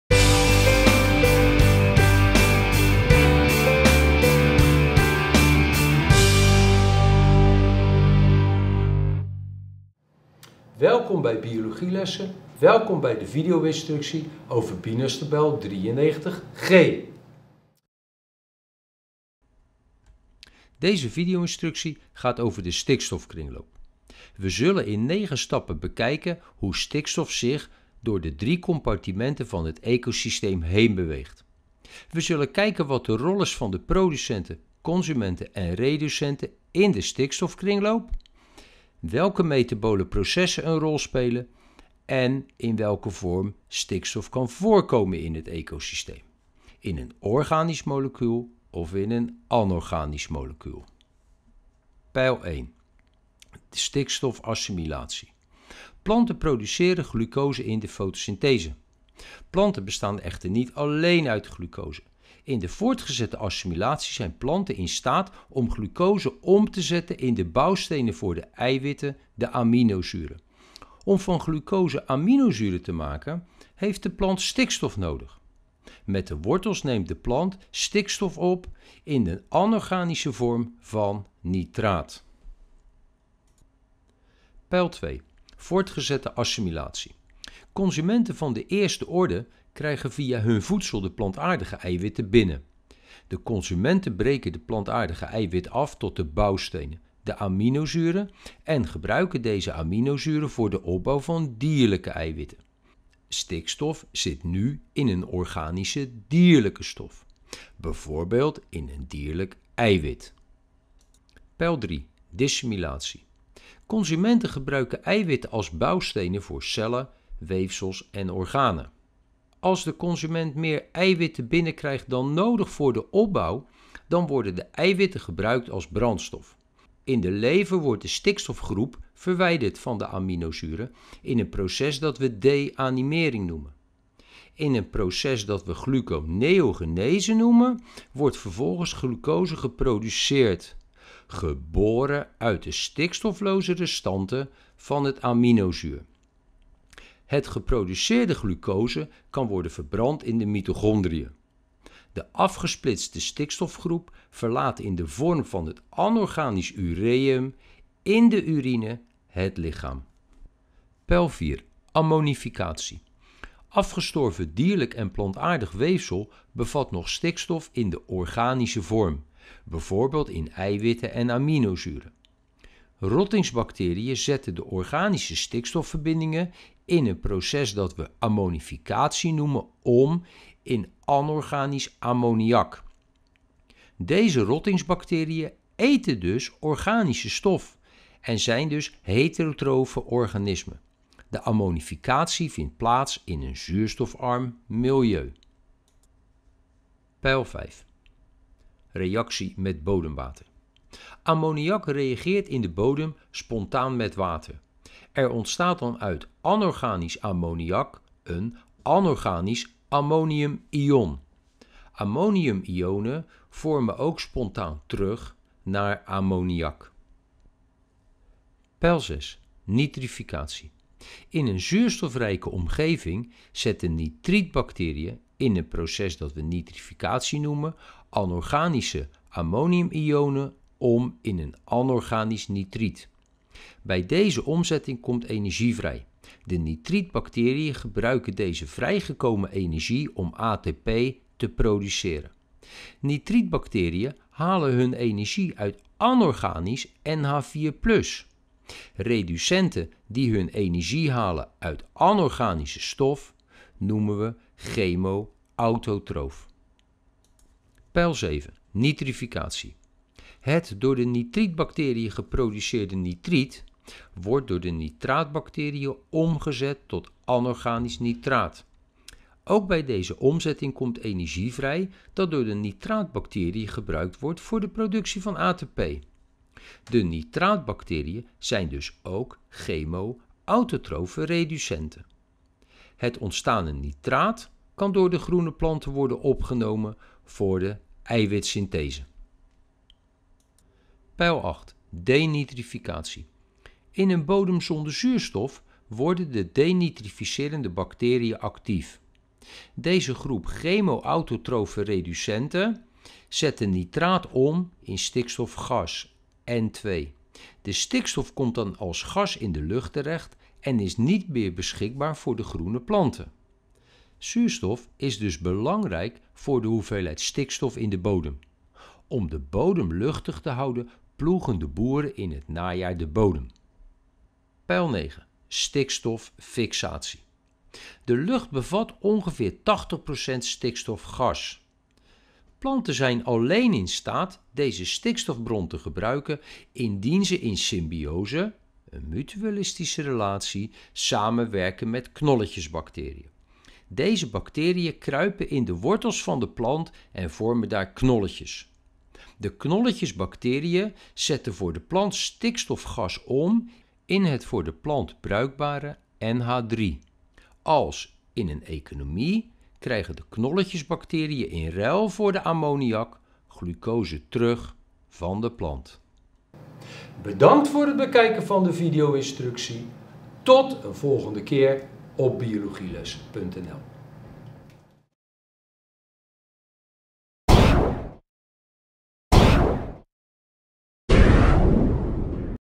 BiNaS-tabel 93G
In deze video bespreken we tabel 93G van BiNas. In deze video wordt besproken door welke compartimenten stikstof stroomt in het ecosysteem. Wat de rol is van de producenten, consumenten en reducenten in de stikstofkringloop. Ook worden de verantwoordelijke metabole processen besproken.